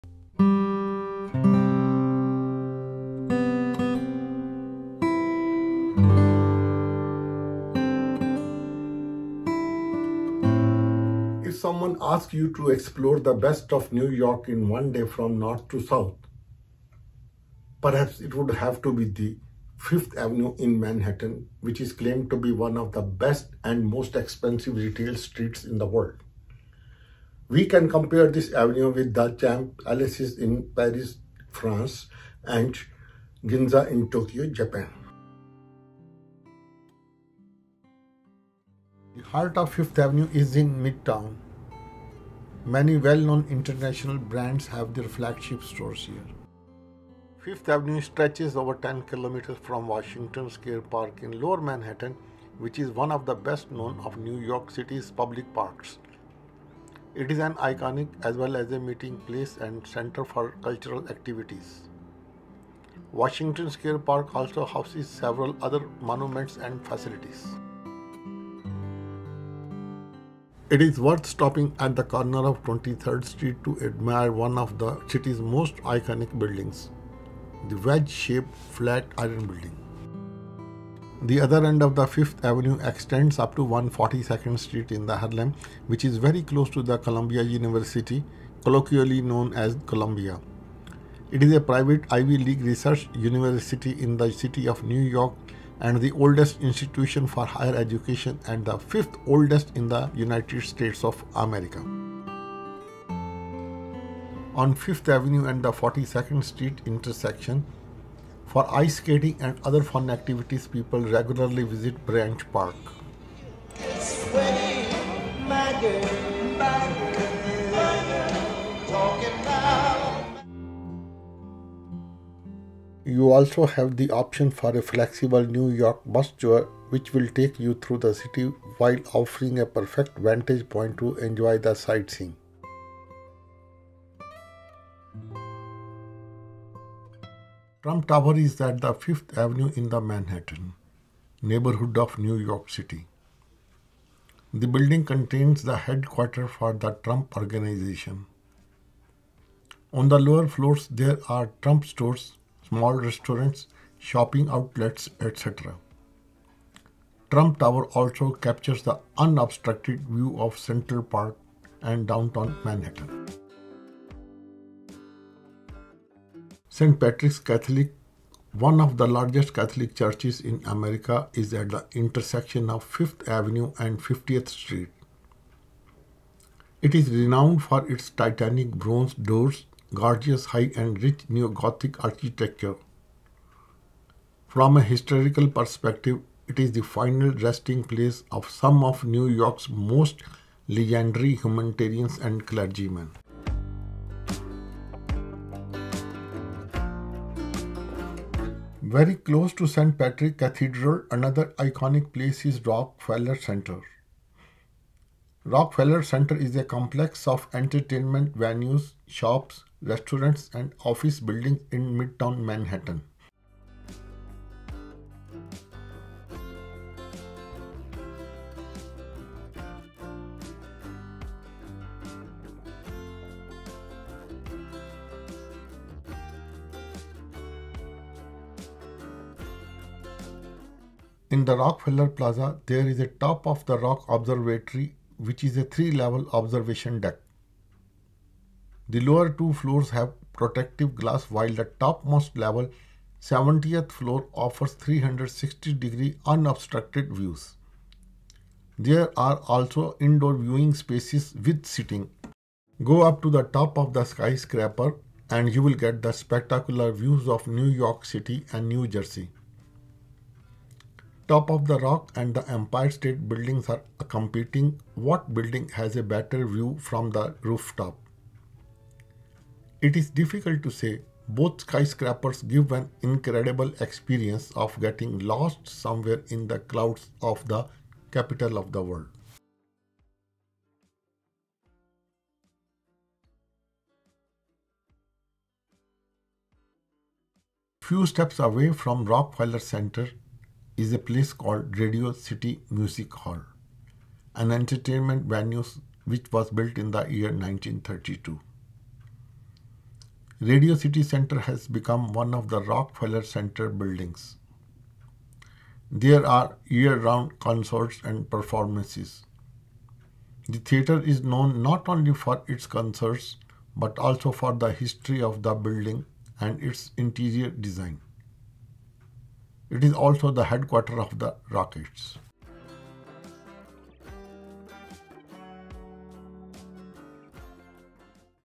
Uncover the Charms of Fifth Avenue: The Soul of Manhattan Revealed
Fifth  Avenue - The heart of Manhattan New York city is  a legendary thoroughfare in the heart of Manhattan, stands as an emblem of elegance and opulence. Stretching from Washington Square Park to the steps of the Metropolitan Museum of Art, this iconic avenue is a mesmerizing blend of architectural marvels, high-end boutiques, and cultural landmarks. Glistening flagship stores of renowned luxury brands line the sidewalks, drawing fashion enthusiasts and trendsetters from around the globe. The street pulses with energy, as yellow taxis weave through a sea of pedestrians, creating a dynamic urban symphony. Tree-lined medians provide a touch of greenery amidst the towering skyscrapers, and Central Park's southern border offers a serene escape. Whether exploring the Museum Mile, admiring the historic St. Patrick's Cathedral, or simply strolling past the exclusive addresses, Fifth Avenue encapsulates the essence of New York City's glamour and sophistication.
walking tour is an exciting experience .  You come across many iconic buildings like Trump Tower, St.Patric Church and many more.

* Camera used: Nikon D7000 and Iphone12 Pro Max.
* Voice recording with Blue Yeti microphone

Twitter.               :  @sainiavinash
Linkdin.               :  Avinash-saini-2488b716/2488b716/